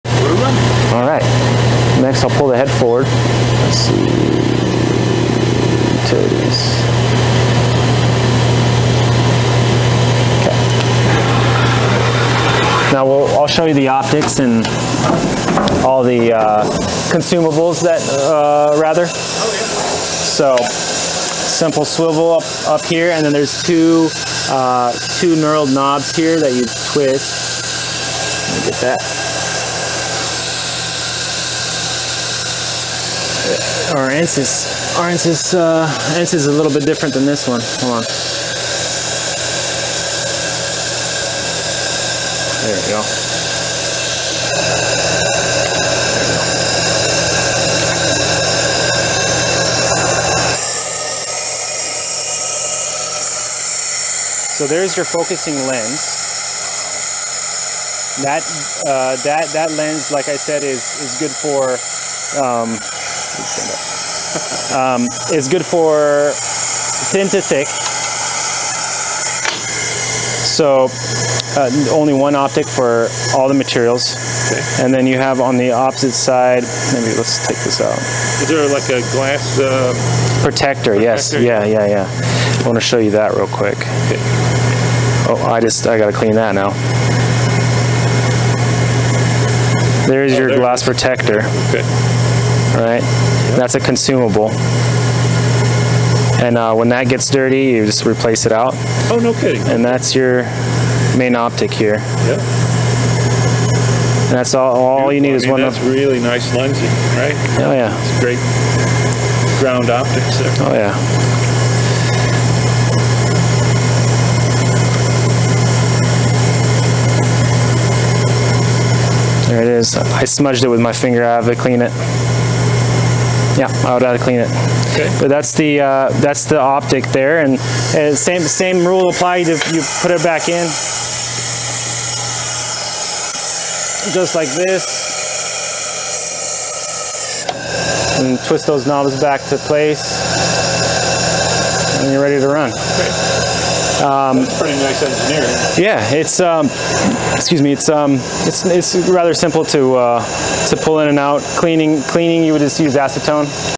AMADA BREVIS #6 Removing the lens
In this video we take out the optics from the head. A lens plus protective glass are crucial parts to the cutting process, and we learn how to remove the assembly from the cutting head and replace it.